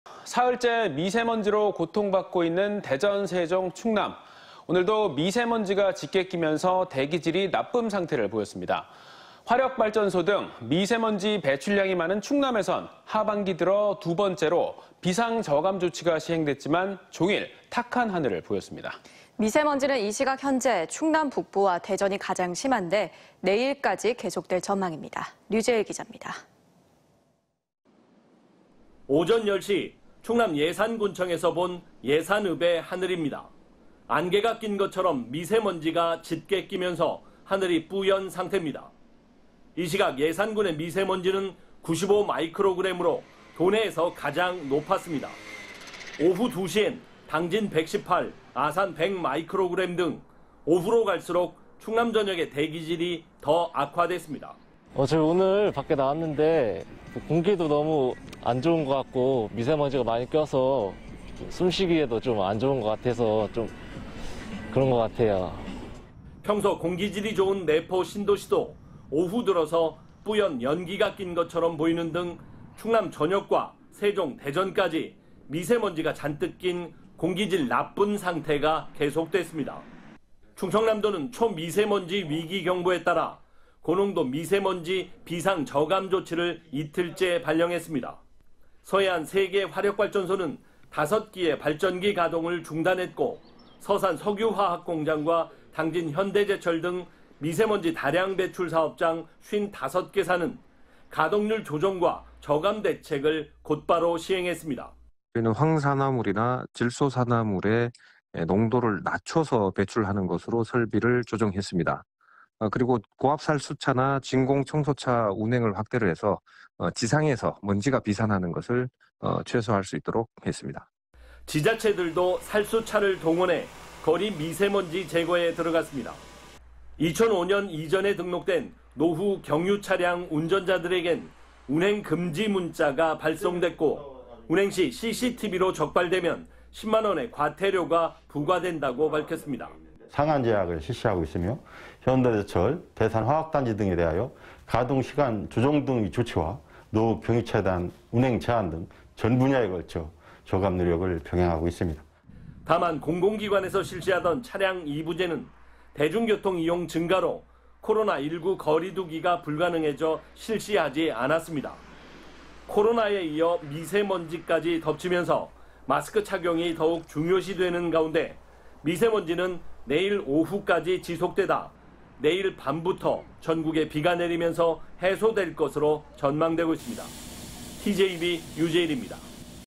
[TJB 대전·충남·세종뉴스]대전·충남 하늘은 '잿빛'..미세먼지까지 고통
사흘째 미세먼지로 고통받고 있는
대전, 세종, 충남에서
오늘도 미세먼지가 짙게 끼면서
대기질이 '나쁨' 상태를 보였습니다.

화력발전소 등 미세먼지 배출량이
많은 충남에선 하반기 들어
두번째로 비상저감조치가
시행됐지만, 종일 탁한 하늘을
보였는데요.

미세먼지는 이 시각 현재 충남 북부와
대전이 가장 심한데
내일까지 계속될 전망입니다.

류 제일 기자의 보도합니다.